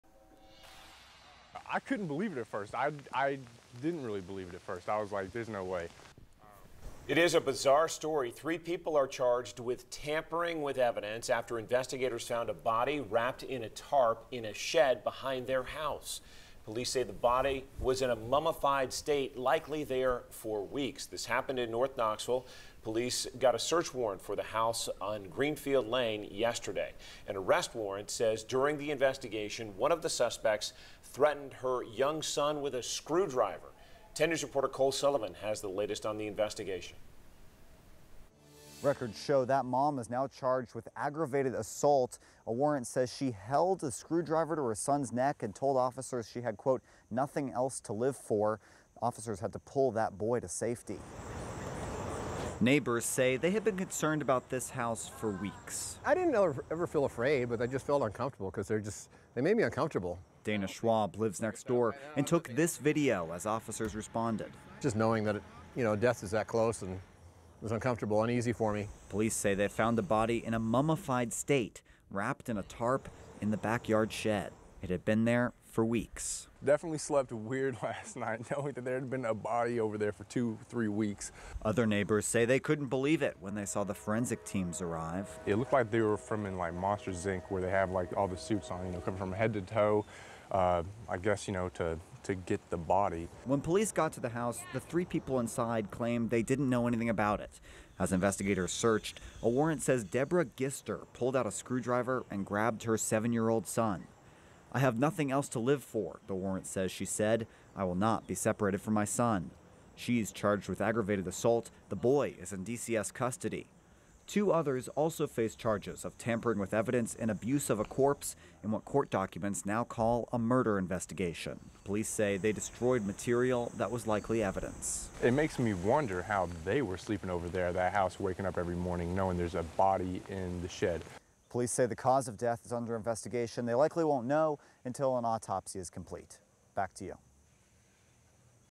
Report: Mummified body being investigated as murder
Three people are charged with tampering with evidence after investigators found a body wrapped in a tarp in a shed behind their house.